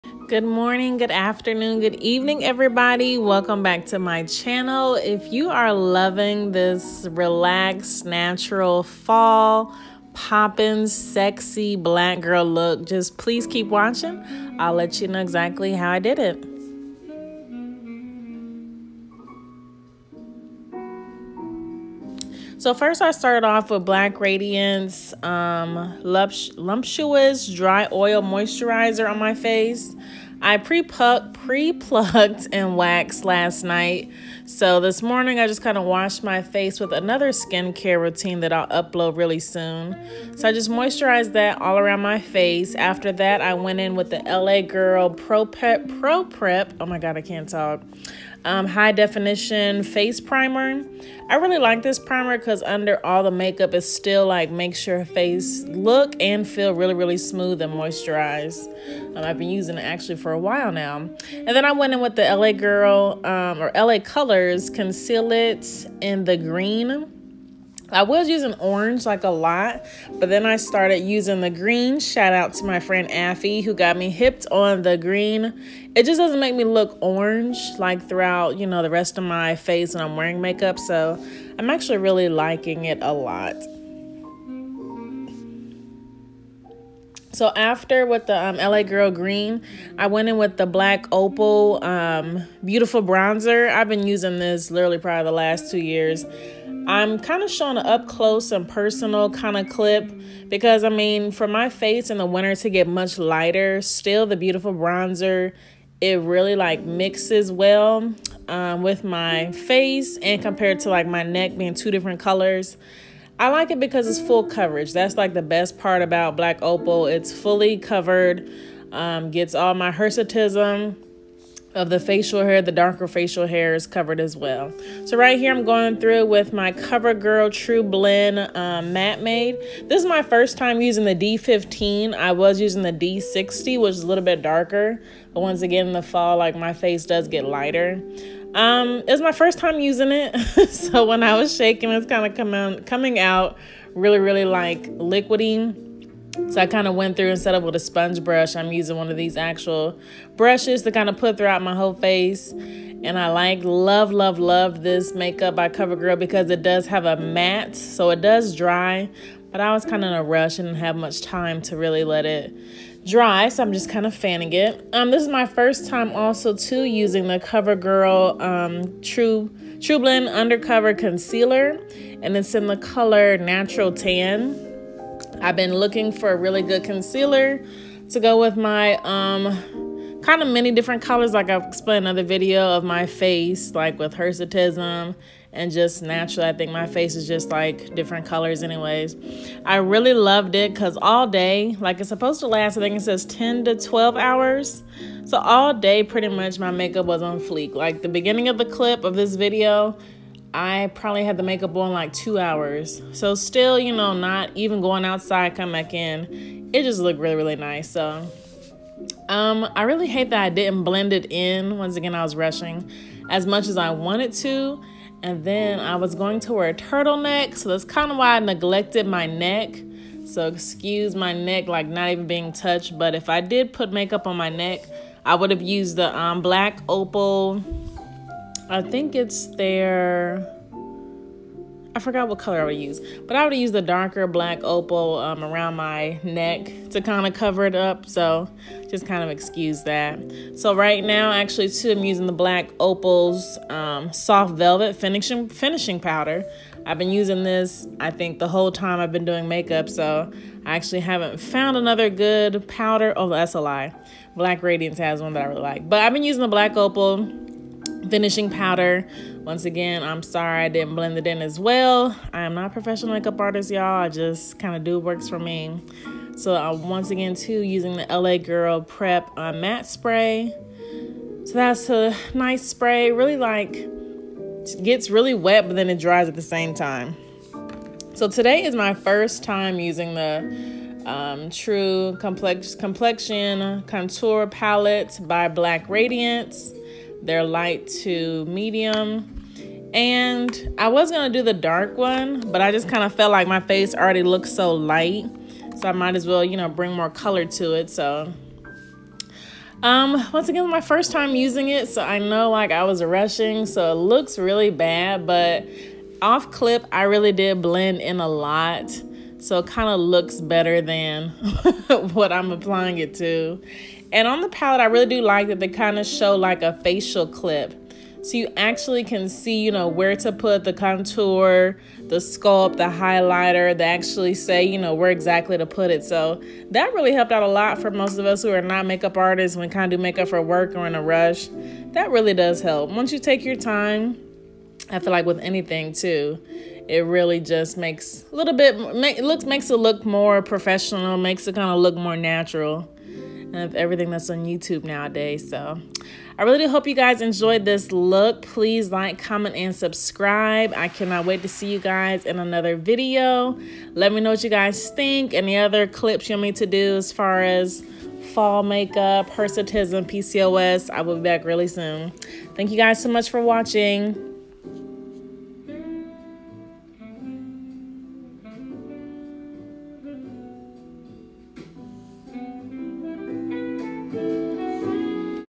Basic 2 Beauty:Fall Makeup for Hirsutism/ PCOS
Heyyyyy Cysters🙋🏾‍♀️💙

Fall season is here and my face is changing colors on top of dealing with PCOS & Hirsutism 😒
I wanted to show new makeup I got for my “Natural/ Everyday” look!
I’m sorry I haven’t posted in a while but looking forward to seeing you guys real soon in more Up coming videos!!

Products Used/ Listed how they appear in video:

*BLACK RADIANCE luminous dry oil

*LA GIRL pro prep face Primer

*LA COLORS conceal it GREEN

*BLACK OPAL Beautiful Bronzer foundation

* COVERGIRL TRUBLEND MATTE MADE foundation D15

*COVERGIRL TRUBLEND UNDERCOVER CONCEALER
T500 Natural Tan

*BLACK OPAL TRUE COLOR
Soft velvet finishing powder

*LA GIRL finishing Matte spray

im far from knowing ANYTHING about Makeup; I just do what works best for me...Please forgive me if i do anything wrong with the brushes, blending and using make up .